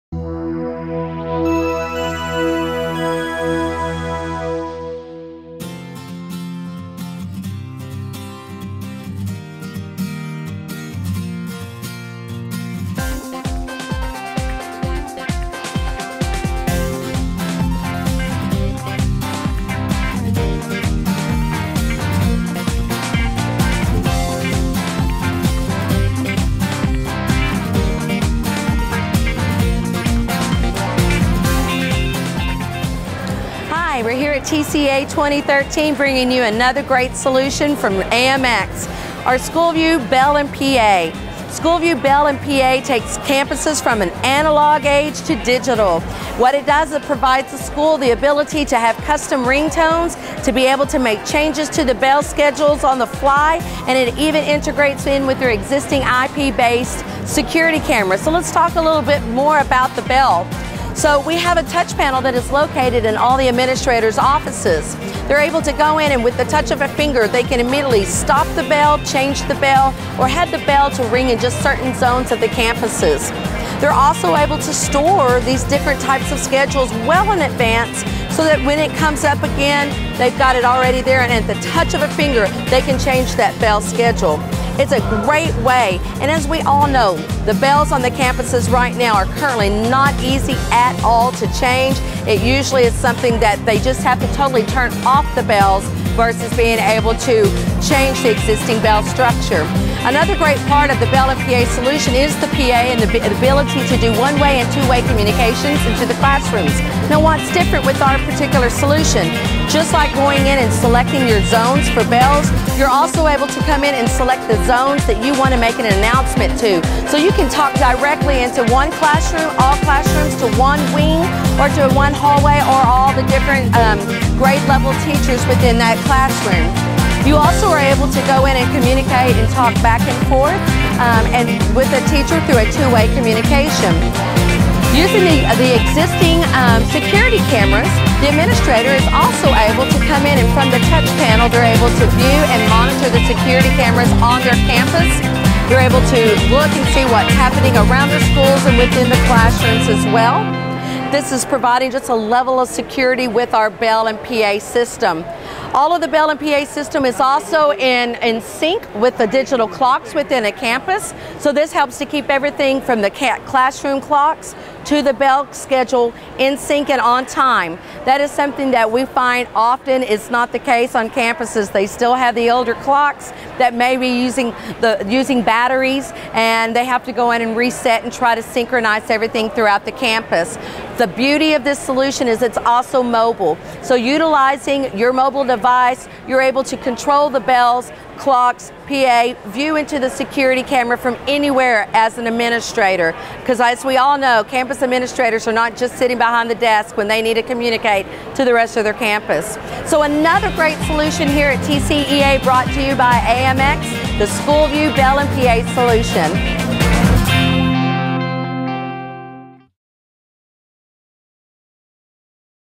Unified Campus Solution: Bell & PA
The SchoolView Bell & PA system consolidates multiple systems including bells, PA/Intercom, remote audio, background music, and security camera feeds into a simple, user-friendly interface over a single network. Who says you can't unify your campus technology?

For more information on the AMX SchoolView ResQ product go

To download your complimentary planning guide for keeping your campus safe and secure go